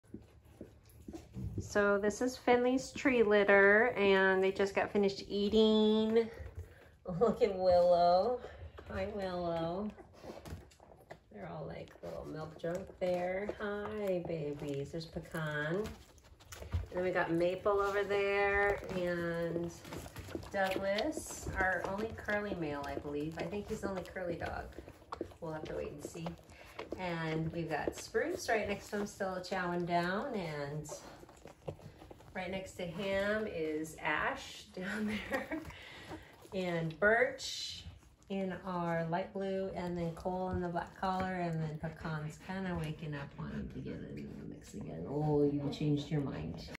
Finley’s 2-week old Goldendoodle puppies
Milk-drunk puppies!
Finley & Duke’s 2nd litter of medium multigen Goldendoodle puppies turn two weeks old and now have started opening their eyes!